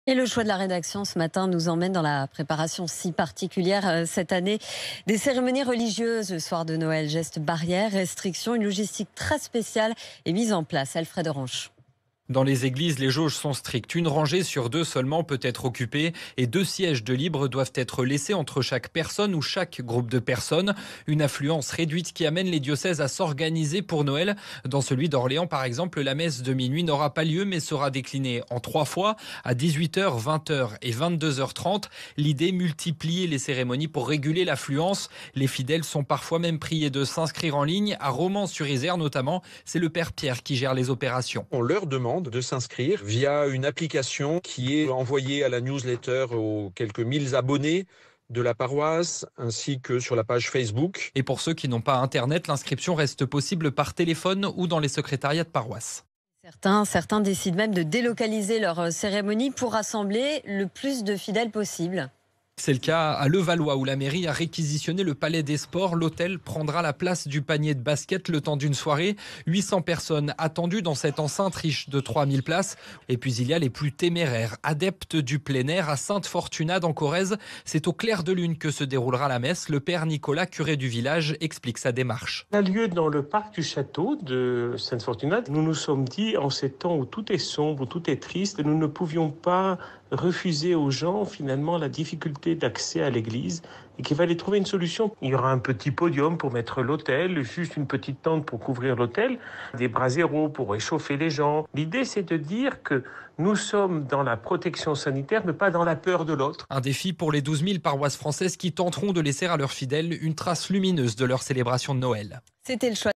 Messe de Noël: une logistique très spéciale sera mise en place cette année
Messe de Noël: une logistique très spéciale sera mise en place cette année. Gestes barrières, restrictions sanitaires, une logistique très spéciale est mise en place